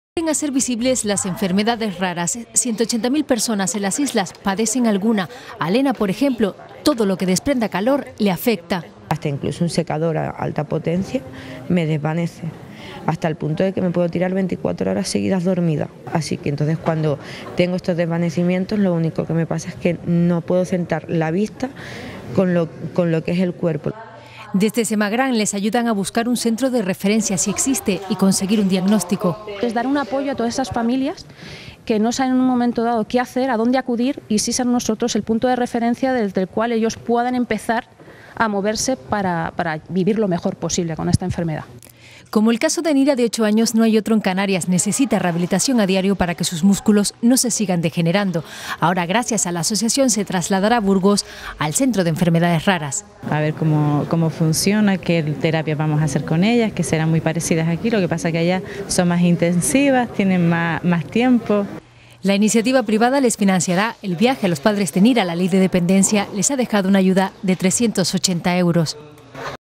Enfermedades raras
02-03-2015

VII Jornada de Enfermedades Raras en Canarias

La Concejalía de Salud del Ayuntamiento de Granadilla de Abona, cuya responsable es Guacimara González, te invita a la VII Jornada de Enfermedades Raras en Canarias, que bajo el lema ‘Tenemos Derecho’, se celebra el sábado 21 de marzo en el auditorio del San Isidro Espacio Cívico (SIEC) en horario de 10.00 a 14.00 horas. La iniciativa pretende incidir en la defensa de los derechos de los pacientes y el análisis del cumplimiento de la legislación vigente en materia de enfermedades raras. Para ello se contará con el colegio de abogados de la provincia de Santa Cruz de Tenerife que analizará el cumplimiento de la legislación farmacéutica para la investigación, producción y comercialización de los medicamentos huérfanos.